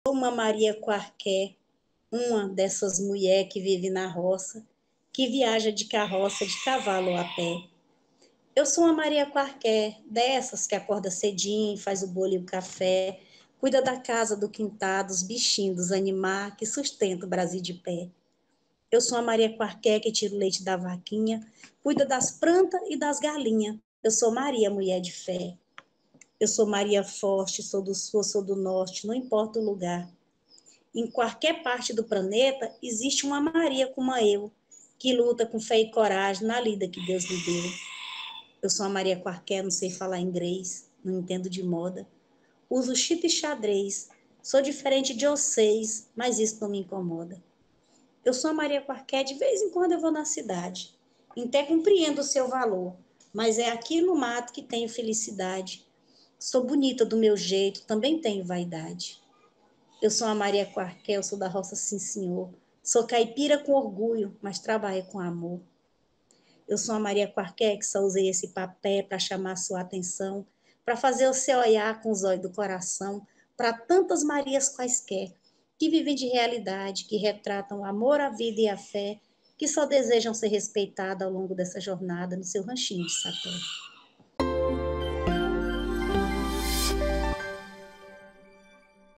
Viva Maria, 40 anos: conheça a poesia de Kennya Silva
Kennya Silva recita a poesia Maria Quarqué.